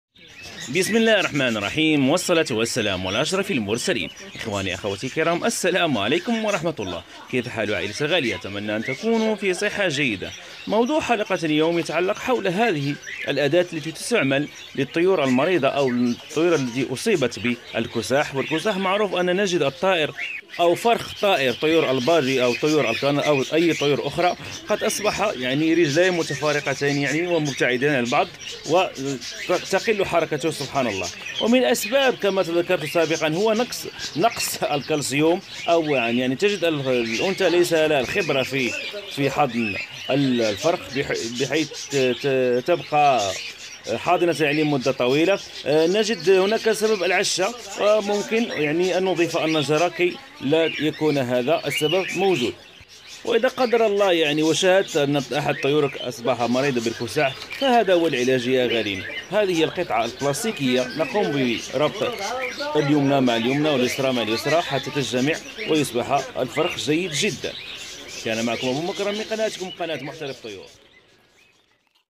علاج مشكل كساح طيور البادجي،الكناري،الزيبرا
من أبرز المشاكل التي تواجهنا في تربية البادجي هي أننا نجد فراخ البادجي اصبح به كساح وذلك من خلال عدم اعطاء الأنثى الكمية الكافية من الكلسيوم او عدم خبرتها في مسألة الحضانة...وغيرها من الأسباب المذكورة فب الفيديو
مشاهدة طيبة وأي سؤال أنا في الخدمة